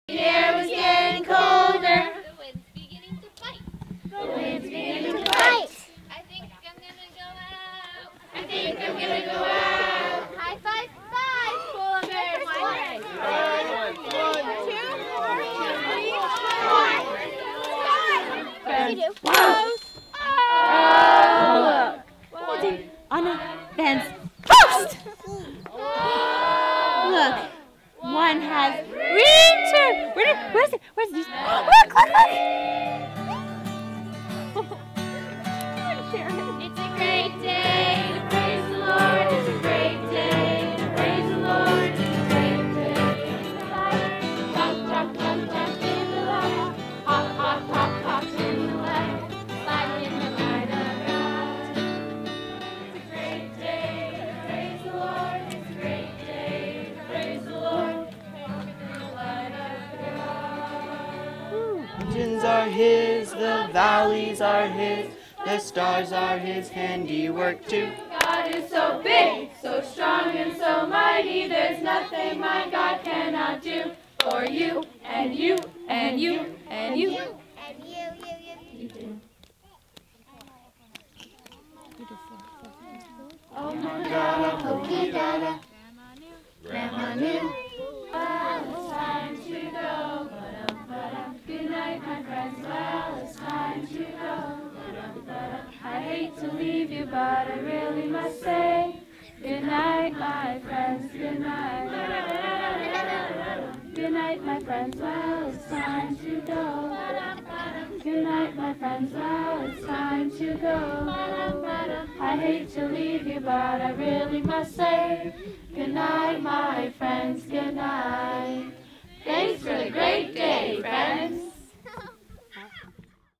Sprouts K5 2013 - Campfire
A video taken at the Monday night campfire at Sprouts 2013.